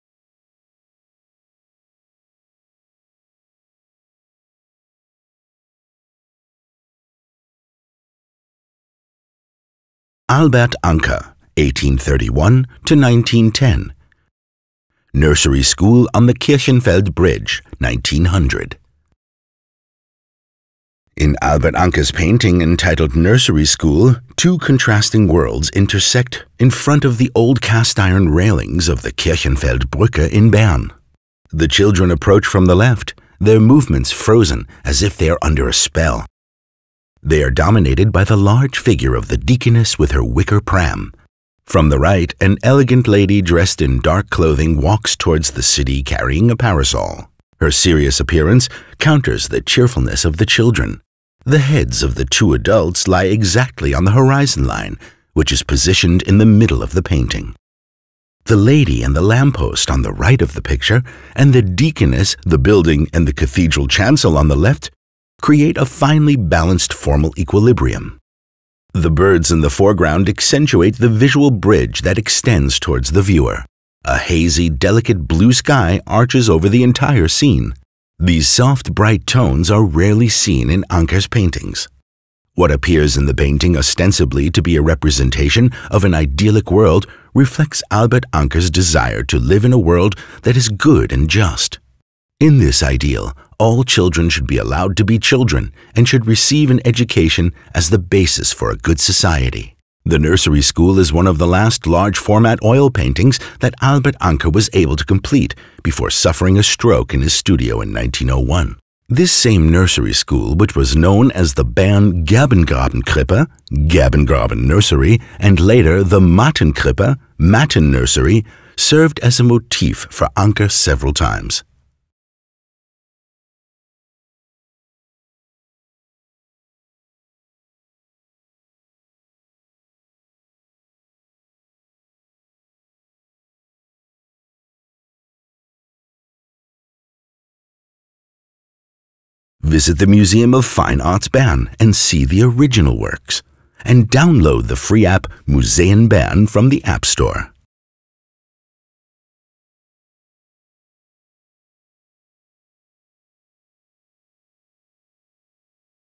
Albert Anker - Nursery School on the Kirchenfeld bridge, 1900
Learn more about Albert Anker's painting depicting the children of a day nursery walking over the Bernese Kirchenfeld bridge. The work is one of this famous artist's last large-format oil paintings. From the podcast series on the highlights of the Kunstmuseum Bern Collection.